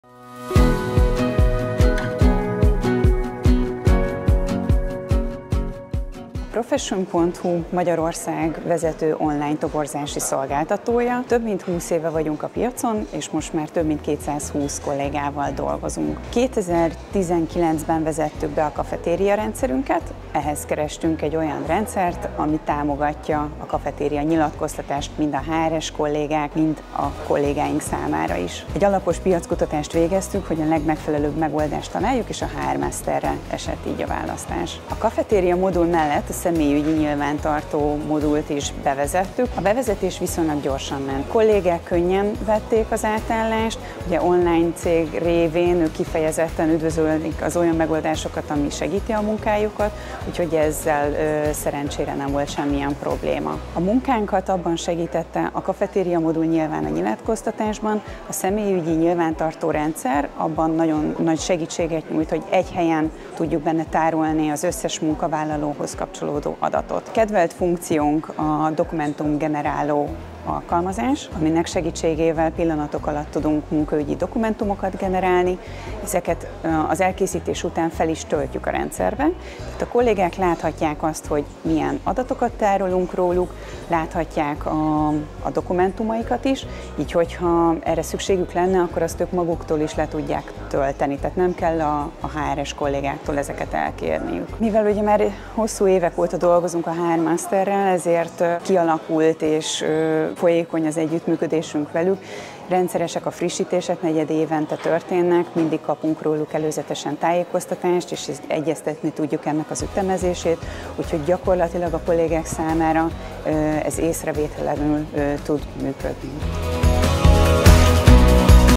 Miben könnyíti meg a HRmaster használata partnereik munkáját?
Miben könnyíti meg a HRmaster használata partnereik munkáját? Az ország vezető online toborzási szolgáltatója, a Profession.hu is a HRmaster moduljaival dolgozik már 2019 óta.

Arról, hogyan zajlott a bevezetés, miben segíti a munkájukat, és az együttműködésről magáról Simon-Göröcs Lili, a Profession.hu HR-igazgatója mesélt!